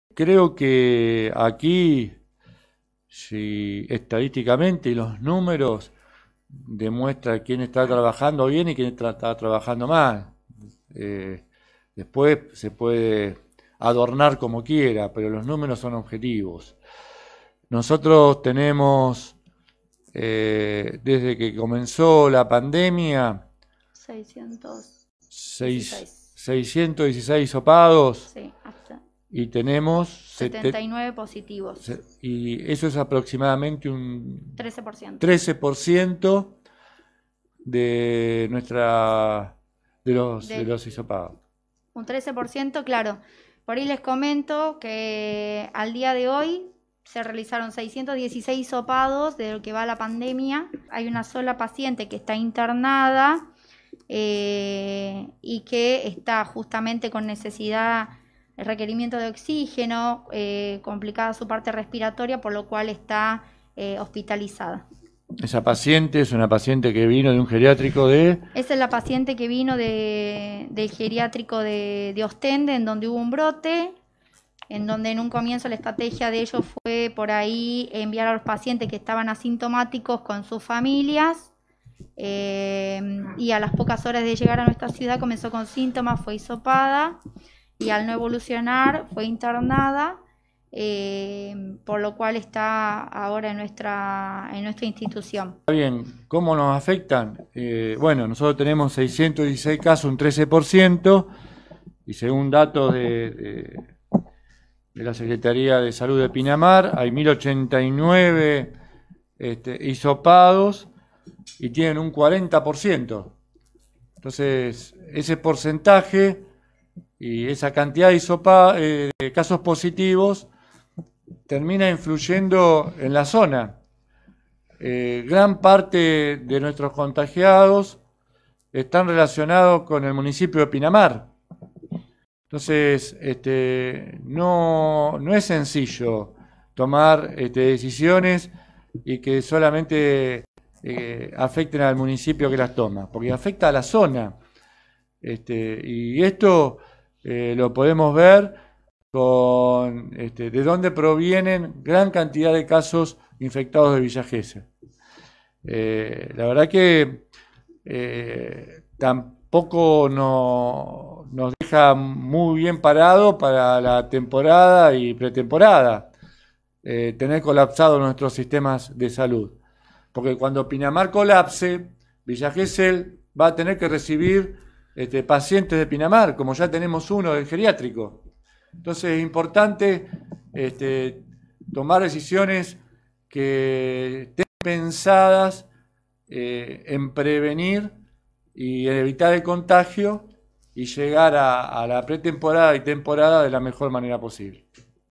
BARRERA CRITICÓ EL MANEJO DE LA PANDEMIA DE PINAMAR
El intendente de Villa Gesell, Gustavo Barrera, alertó sobre el número de casos de Coronavirus de Pinamar y criticó con dureza el manejo de la pandemia de dicha ciudad. "Los números demuestran quién hizo las cosas bien y quién mal. Después lo pueden adornar como quieran", expresó.